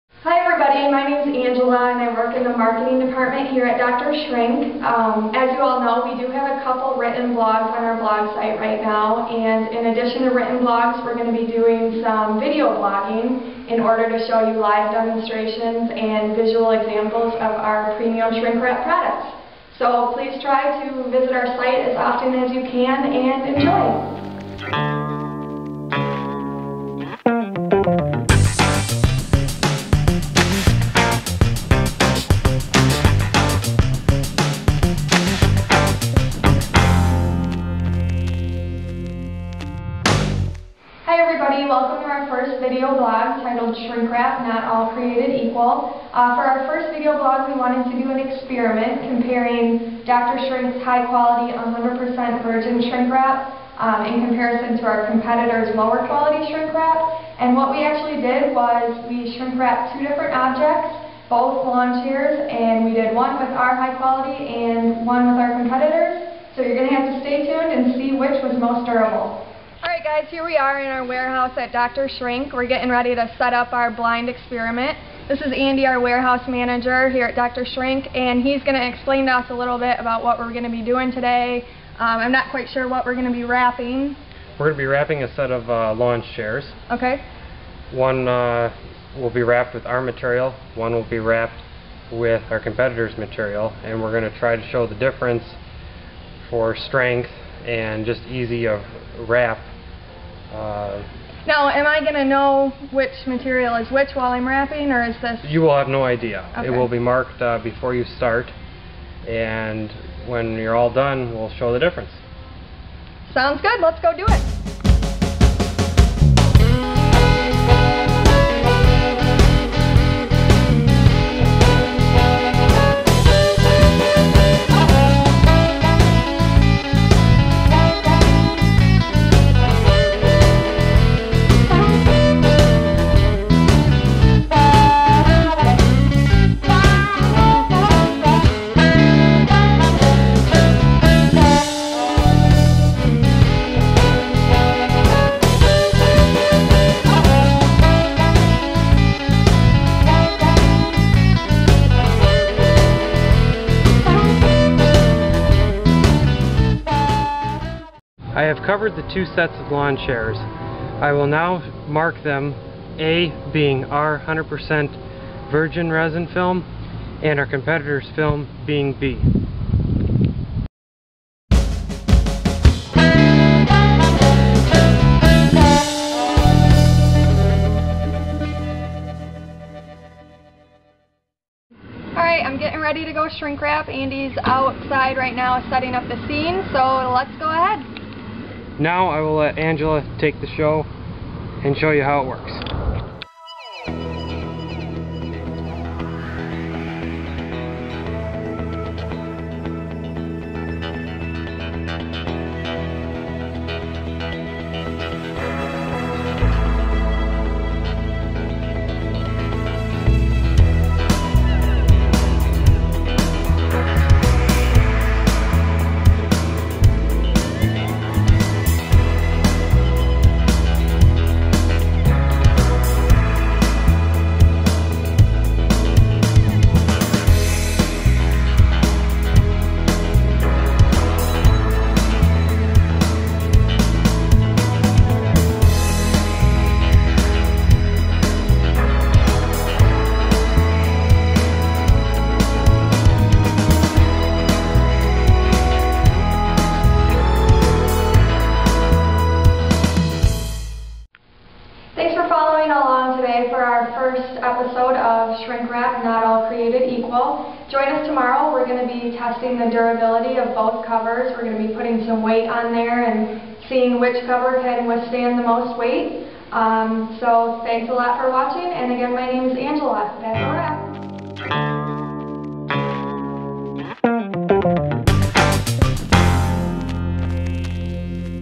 Shrink Wrap: Not All Created Equal, Episode 1
An experiment designed to demonstrate the difference between Dr. Shrink's premium 100% virgin resin shrink wrap and competitors lower quality shrink wrap.